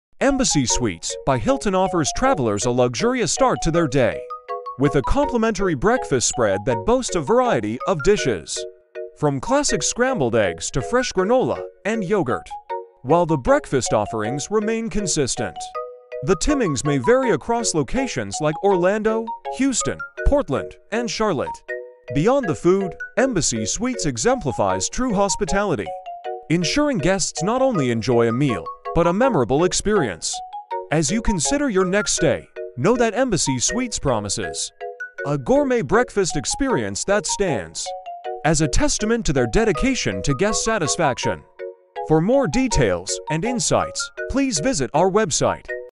Embassy Suites Breakfast Hours
Embassy Suites by Hilton offers travelers a luxurious start to their day with a complimentary breakfast spread that boasts a variety of dishes, from classic scrambled eggs to fresh granola and yogurt. While the breakfast offerings remain consistent, the timings may vary across locations like Orlando, Houston, Portland, and Charlotte. Beyond the food, Embassy Suites exemplifies true hospitality, ensuring guests not only enjoy a meal but a memorable experience. As you consider your next stay, know that Embassy Suites promises a gourmet breakfast experience that stands as a testament to their dedication to guest satisfaction.